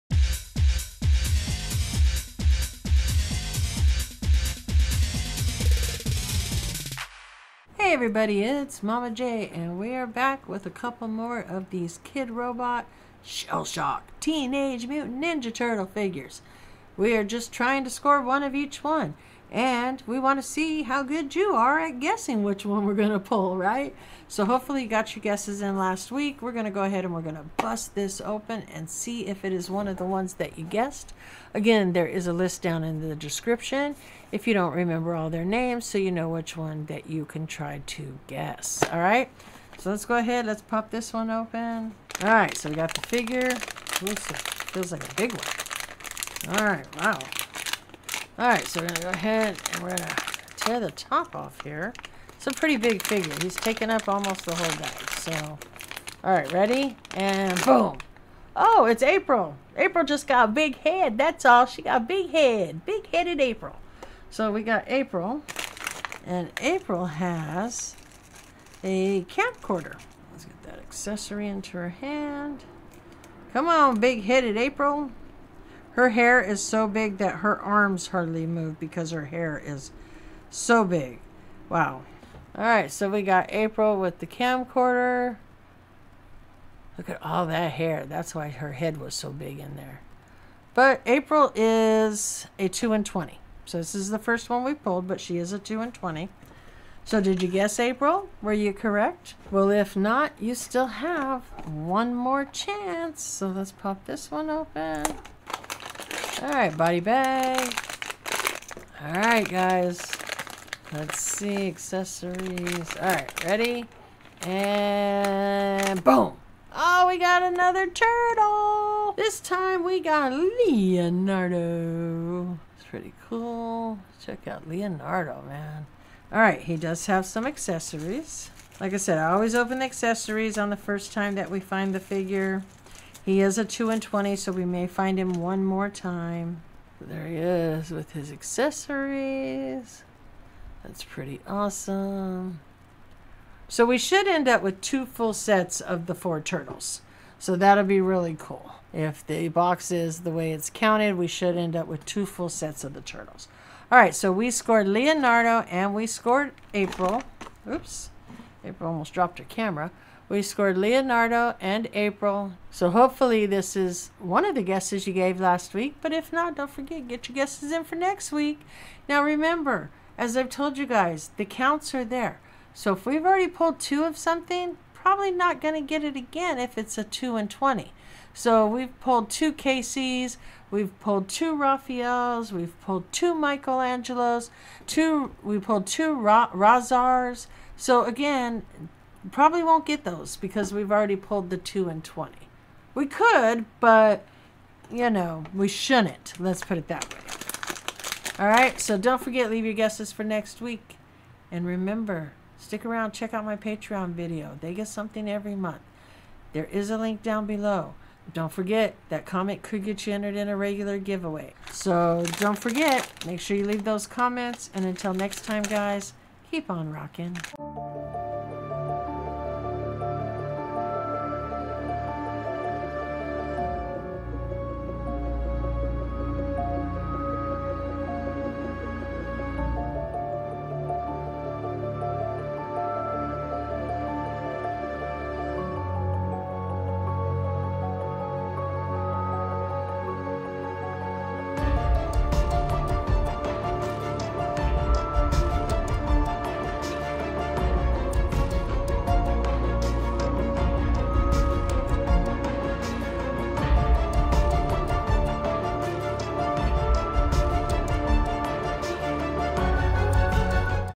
TEENAGE MUTANT NINJA TURTLES Shell Shocked KIDROBOT Vinyl Figures Hunting for them all Part 7
TMNT Teenage Mutant Ninja Turtles Shell Shock Mini Series 2 Kidrobot
The set includes the four brothers (Leonardo, Donatello, Michaelangelo and Raphael); traditional favorites like April O’Neil and Casey Jones; our favorite baddies from TMNT 2: Secret of the Ooze – Tokka and Rahzar; some old school favorites (Ace Duck, Sgt. Bananas, Mutagen Man, Rat King) and two chases that had my inner 10 year old screaming with joy (Triceriton and Metalhead).  Many figures come with their signature accessories or weapons, such as April with her camera and Triceriton with his blaster.

THESE ARE 2/20
Leonardo          Donatello                Michaelangelo       Raphael
April                  Casey Jones           Rahzar

THESE ARE 1/20
Ace Duck          Sgt. Bananas        Mutagen Man        Rat King
Tokka

Triceriton 1/40
special edition "Metal Head" ?/?

IG CHANNELS
@MAMA_J_1970 basic IG